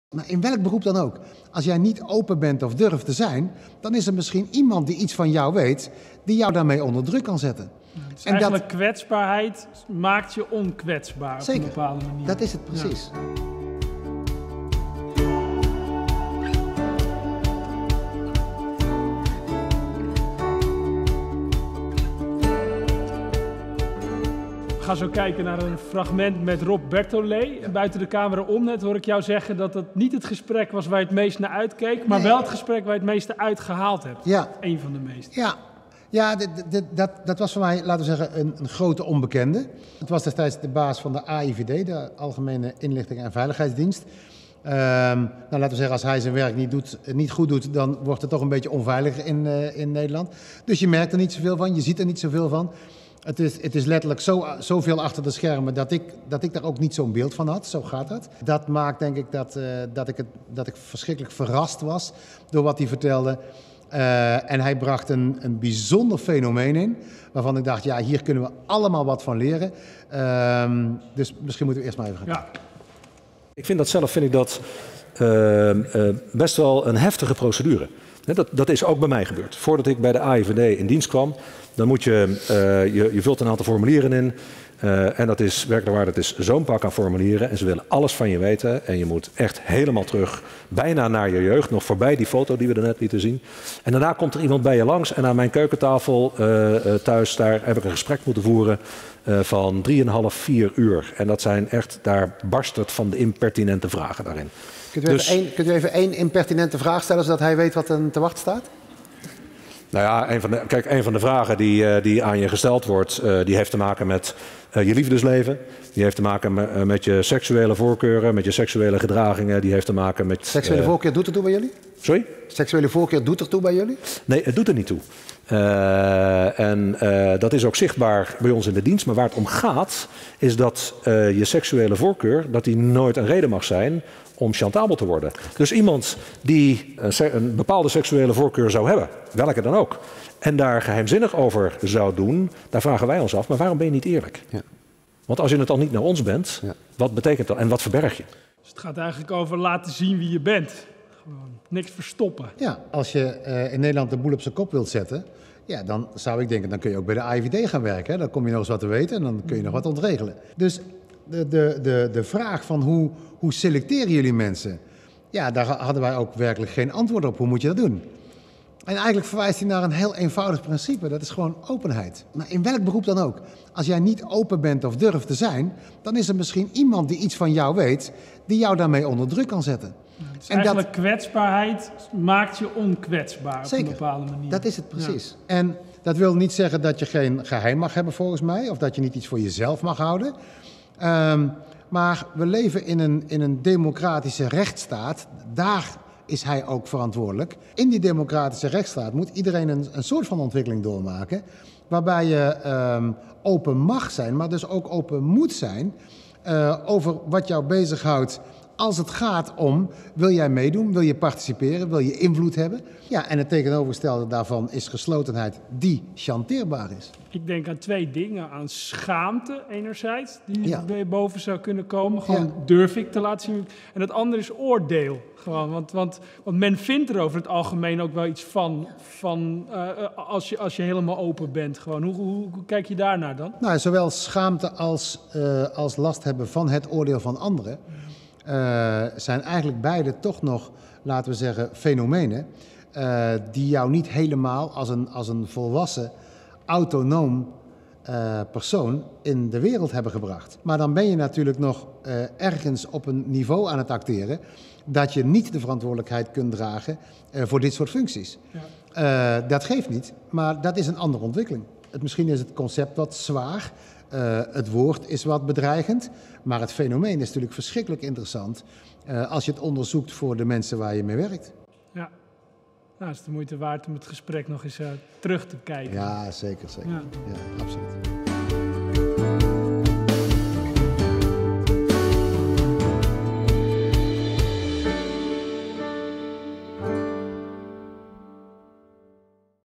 Recht van Spreken: Rob Bertholee
Tijdens de Denk Groter Debatten gaan studenten in gesprek met mensen met recht van spreken. Toppers uit bijvoorbeeld sport, politiek, wetenschap en entertainment die hoogte- en dieptepunten hebben doorleefd. Kwetsbare Held Wout van Wengerden en presentator en pedagoog Marcel van Herpen bespreken in Recht van Spreken de belangrijkste lessen uit de debatreeks. Dit keer: Rob Bertholee.